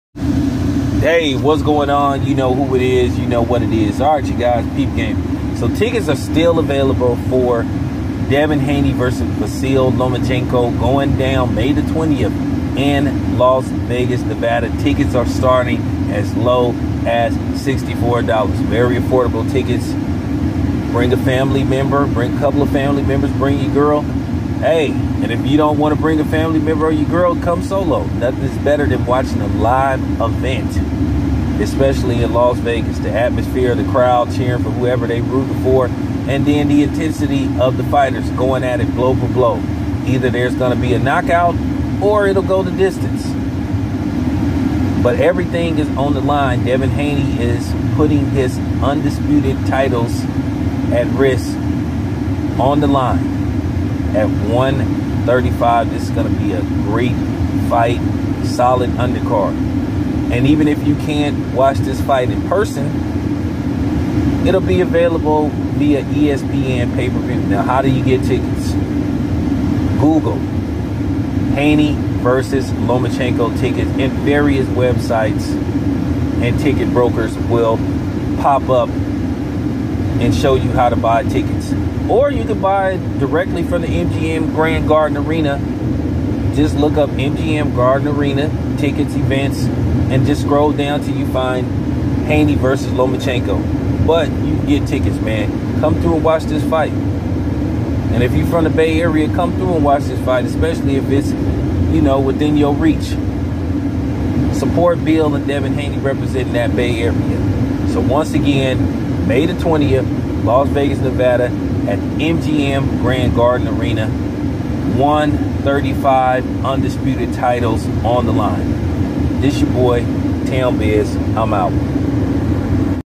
great seats still available for Haney vs Lomachenko
tickets selling for great price for Undisputed Showdown going down May 20th MGM Grand Garden Arena Las Vegas Nevada Haney vs. lomachenko